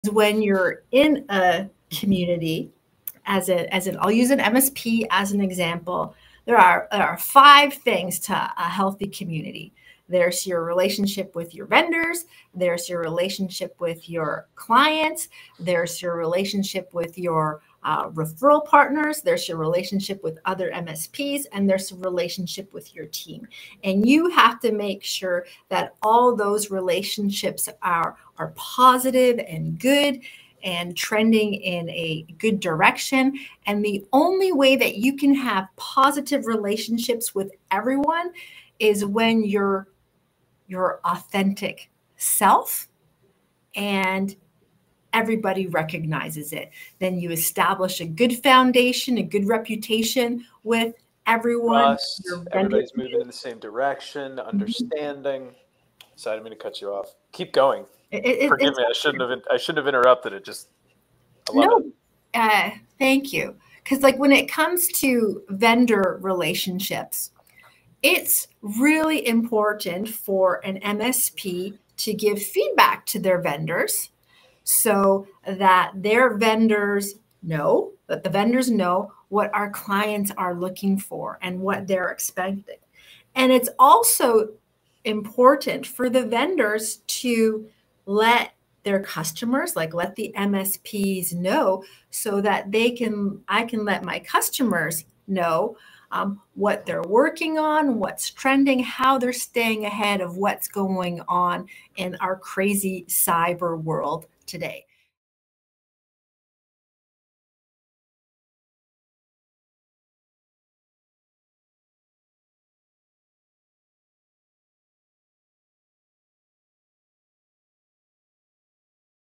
5 Keys to Building Lasting MSP Communities
Barb Paluszkiewicz from CDN Technologies talks about how building lasting communities consists of five key elements. Strong vendor relationships ensure service consistency, while trust with clients fosters loyalty.

Register here to the upcoming MSP Cyber Roundtable about Getting The Ego out of Cyber Expertise